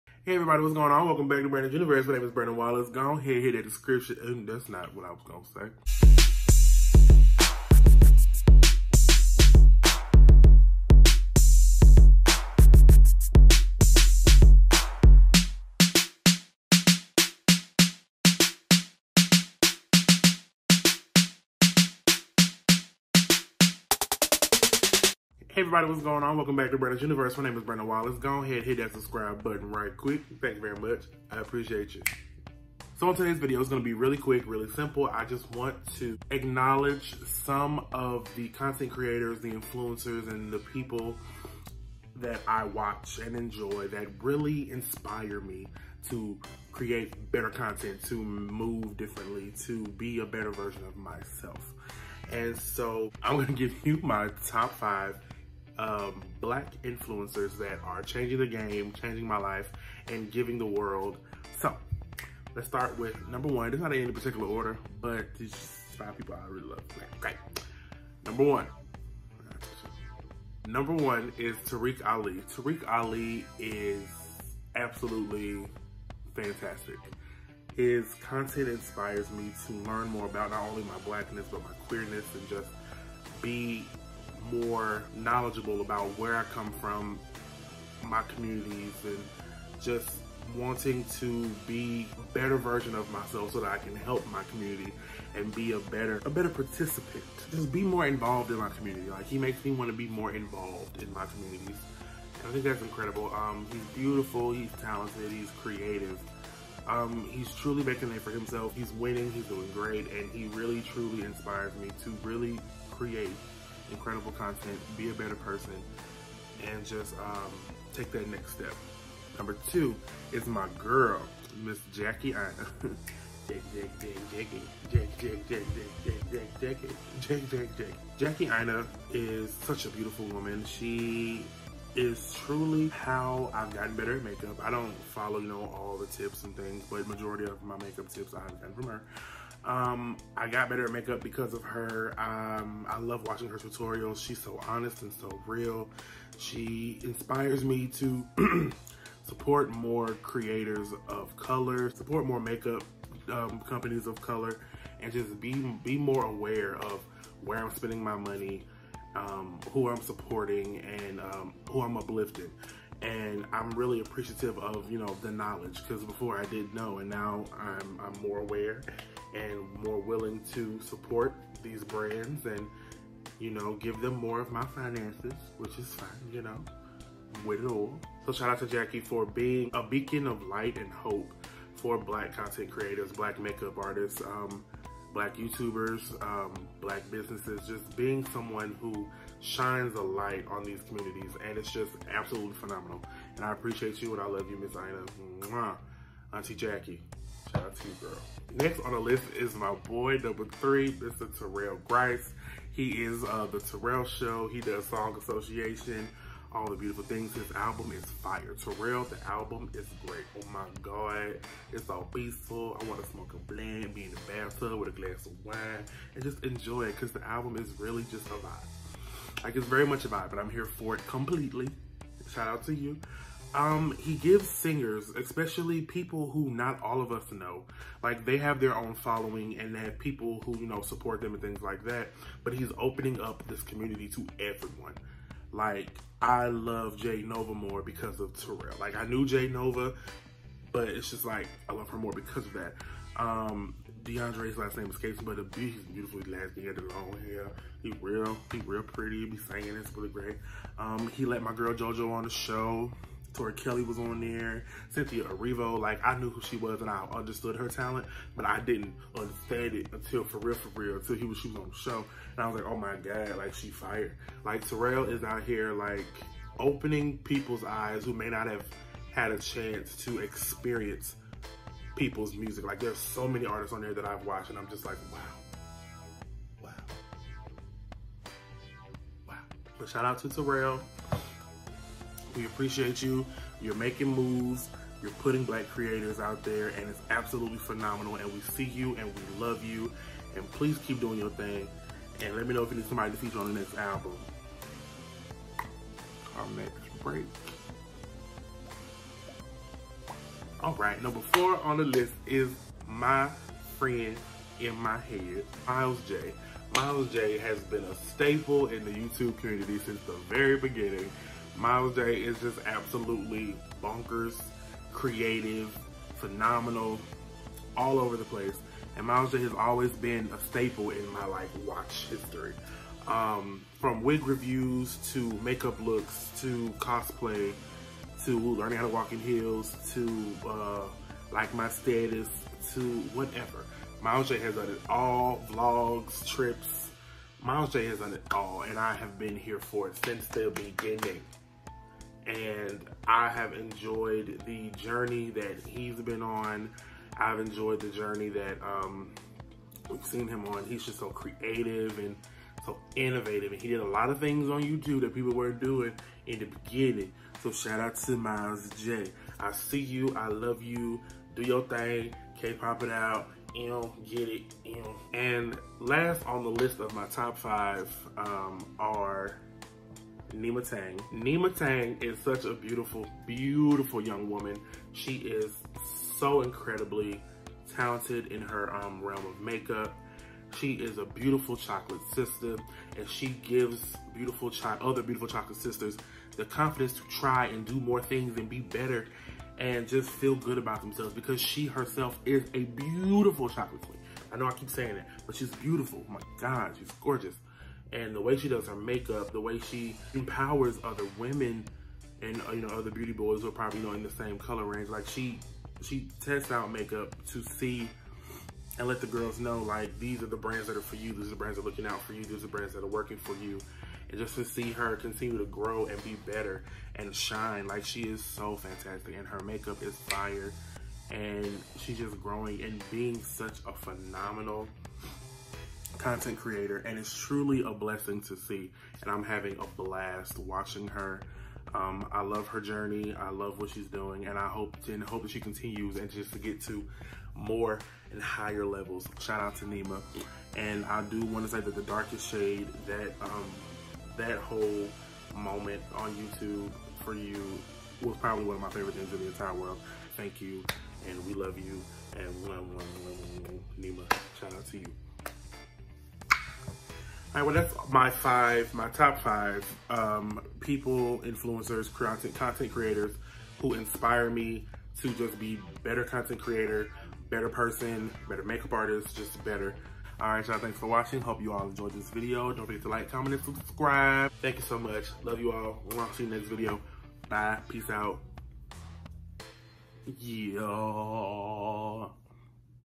Top 5 Black Influencers! ✊🏽✊🏾✊🏿
These folks are just a few of the people that have truly inspired me to be a better content creator! They are helping their communities and truly shining brighter than any other!

Follow on my social medias!